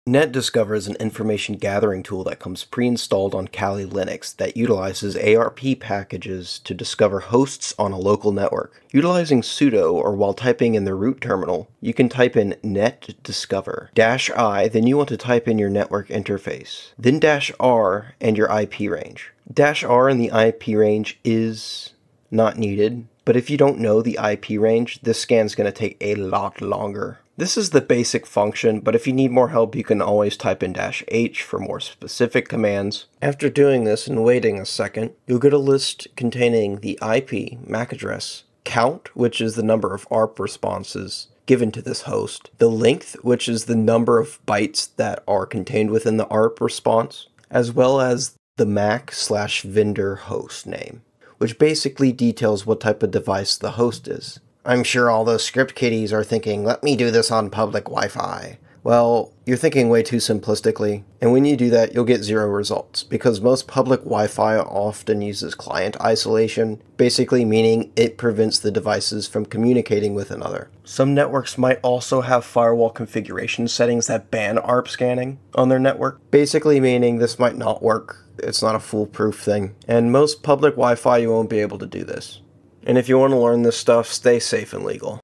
Hack to Find Hidden Devices on Your WiFi Super Fast!
Want to scan networks like a real hacker? Learn how to use NetDiscover on Kali Linux to find devices on your local network using ARP packets. The video covers the basic commands for network scanning, tips for faster scanning, and important cyber security considerations. This is achieved through address resolution protocol. Perfect for ethical hacking, penetration testing, and cybersecurity.